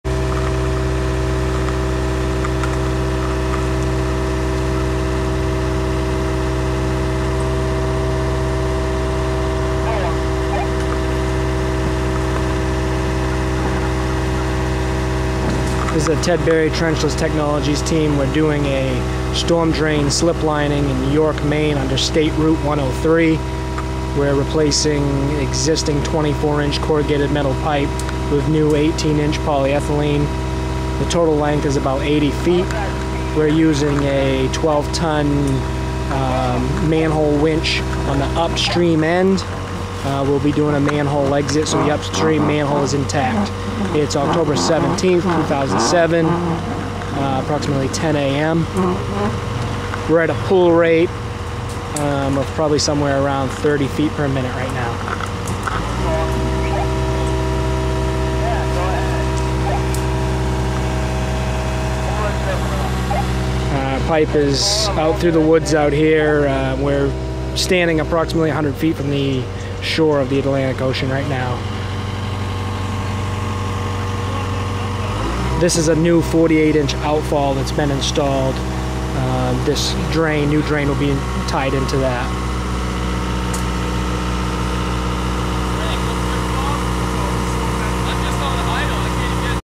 Storm Drain Trenchless Replacement Ted Berry Maine
This shows the Ted Berry Trenchless Technologies Team sliplining an existing 24" CMP with a new 18" HDPE liner in October 2007. A manhole winch was used to save the upstream structure.